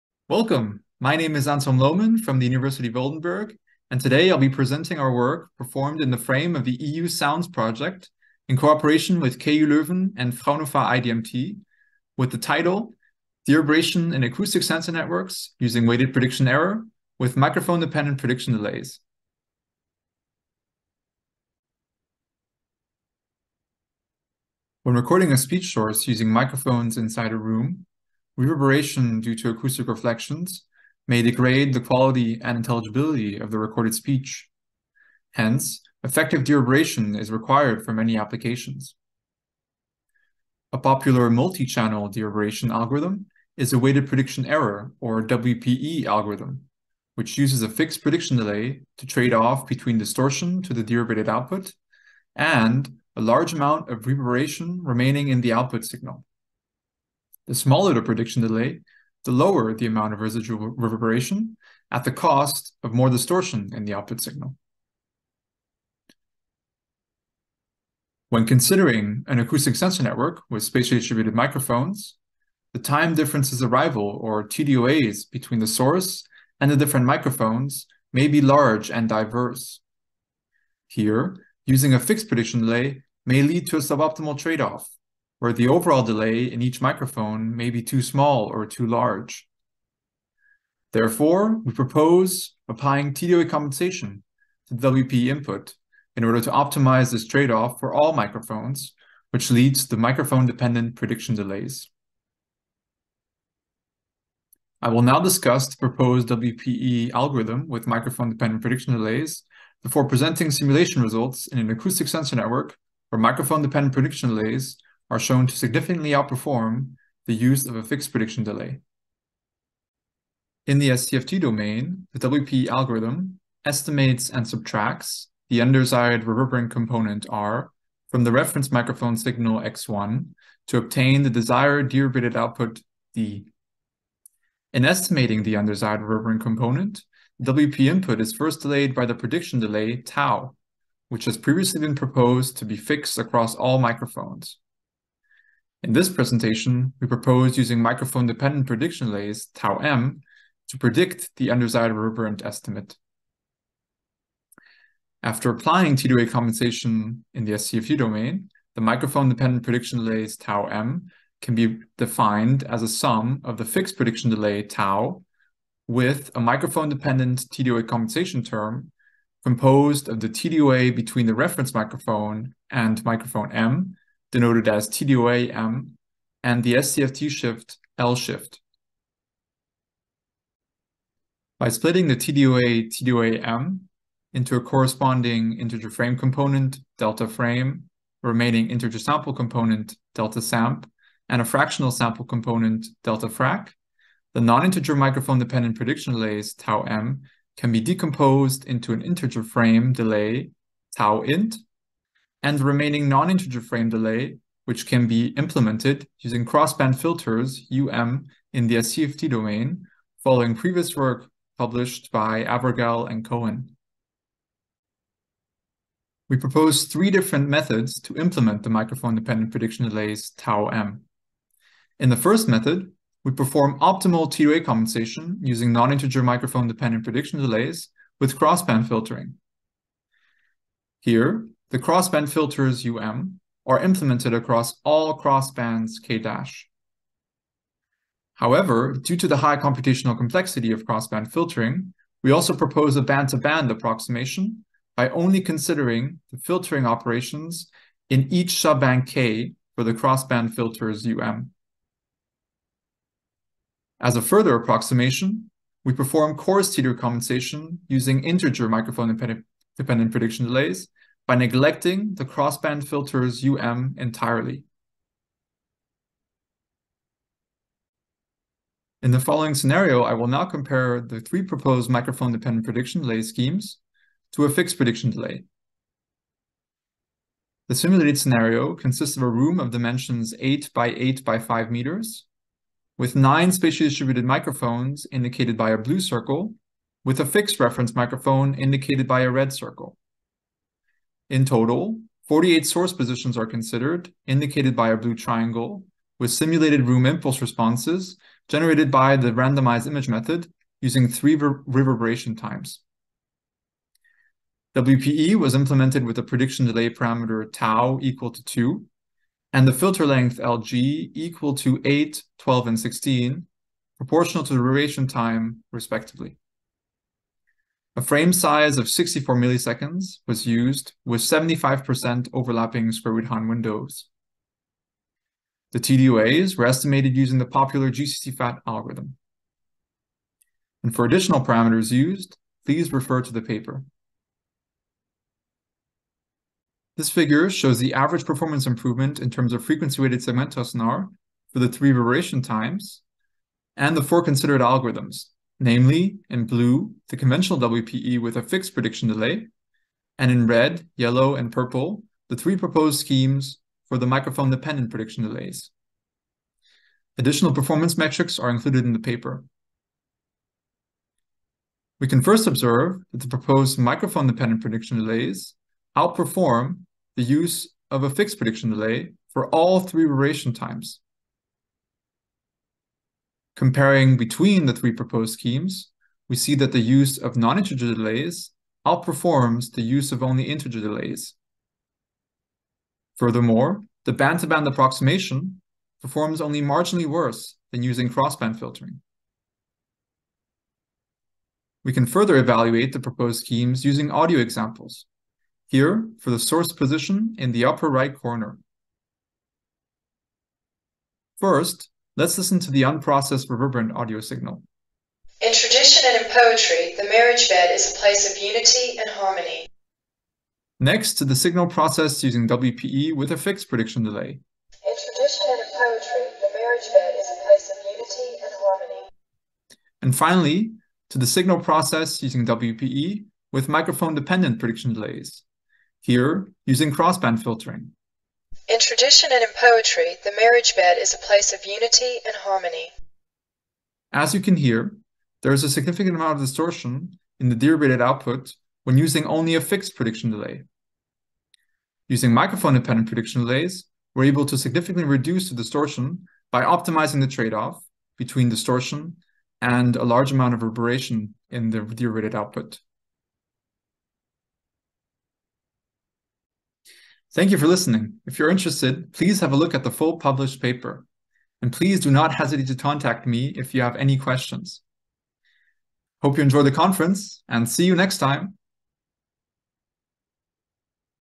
Dereverberation in Acoustic Sensor Networks using WPE with Microphone-dependent Prediction Delays
A. Lohmann, T. van Waterschoot, J. Bitzer, S. Doclo, Dereverberation in Acoustic Sensor Networks using WPE with Microphone-dependent Prediction Delays, accepted for publication in Proc. IEEE International Conference on Acoustics, Speech and Signal Processing (ICASSP), Rhodes, Greece, June 2023.